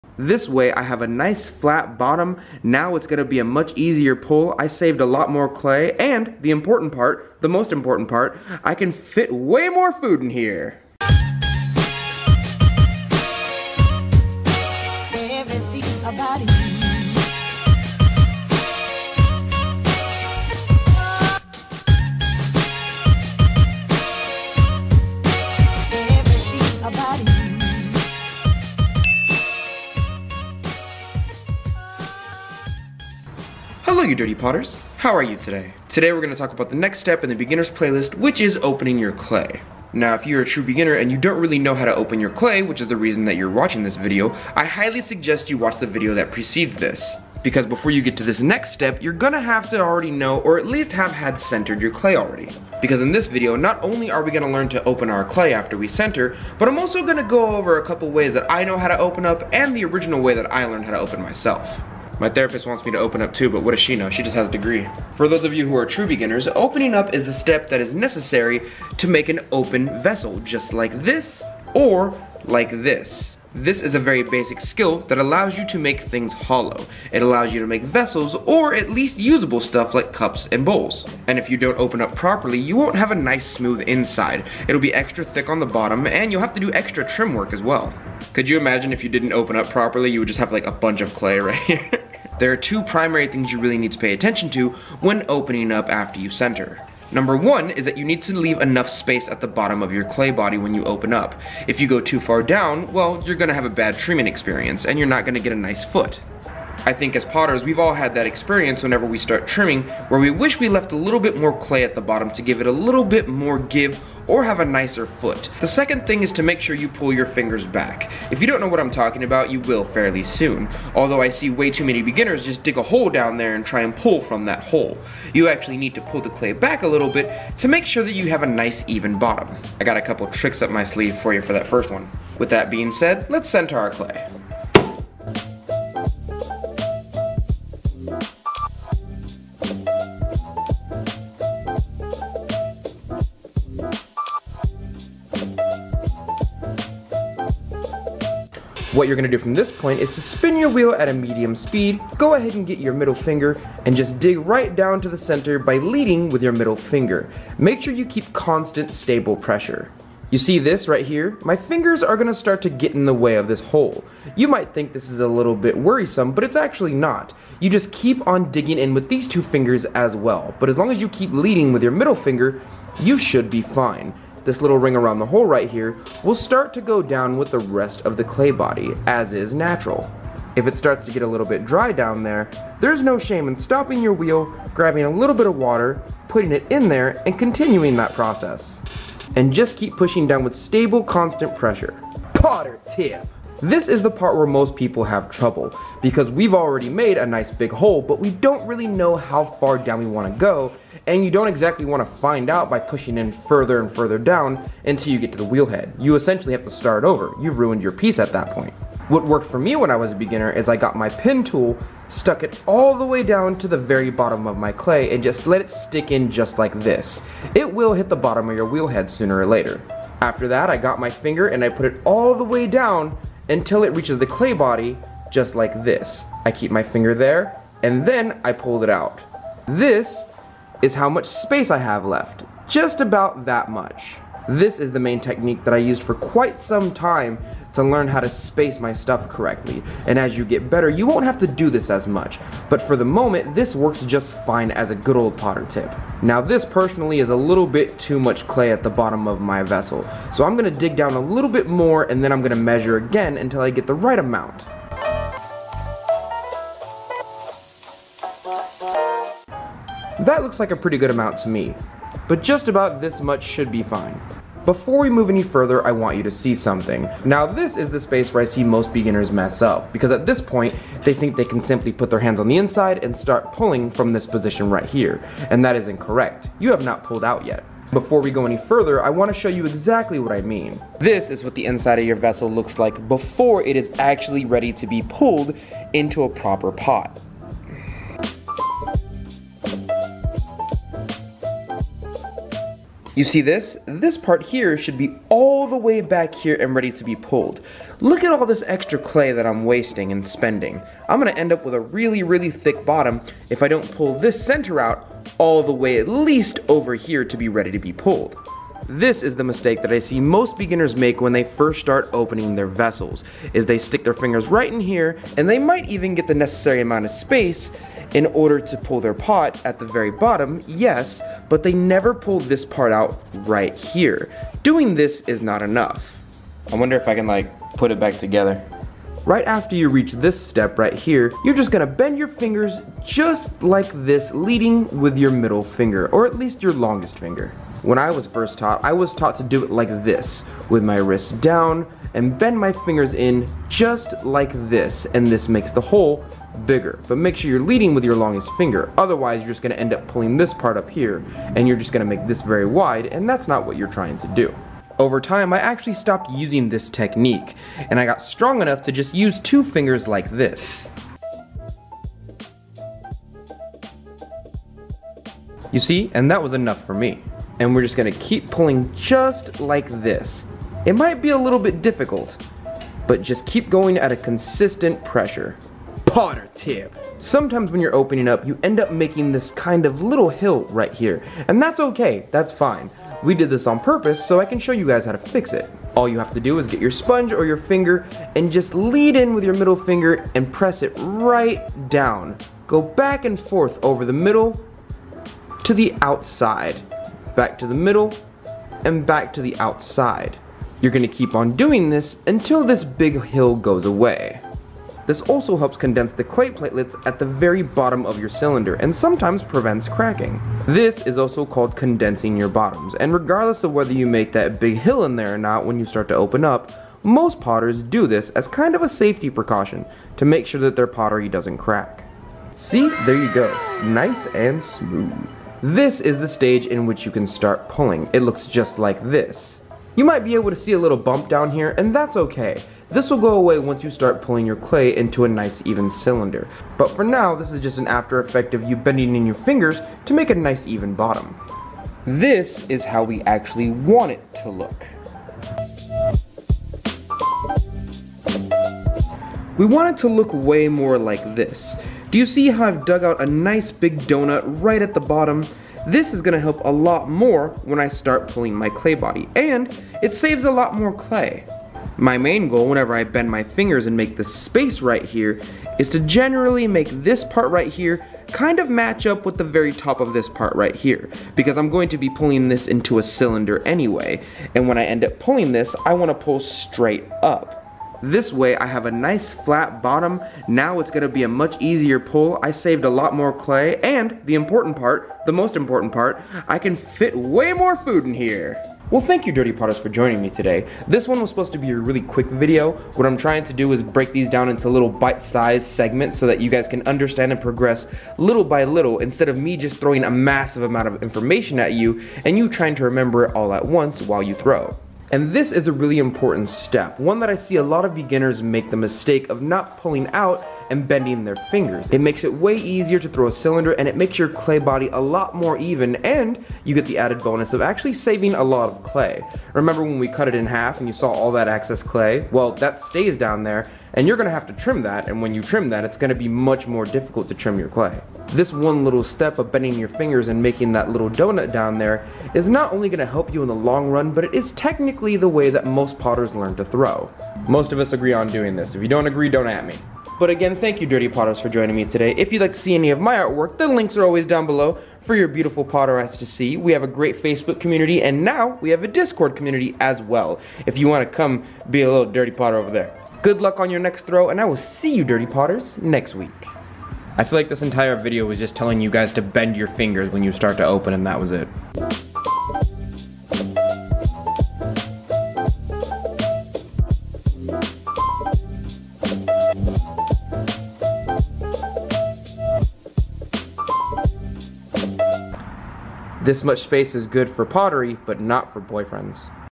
Opening your clay After you center for beginners
This is a very short but necessary video for the progression of the beginners playlist. This video is all about how to open after you center . much like most of what i preach, its all about keeping consistent pressure and being confident in your movements. follow through.

 zip:95823
 name: Donte the potter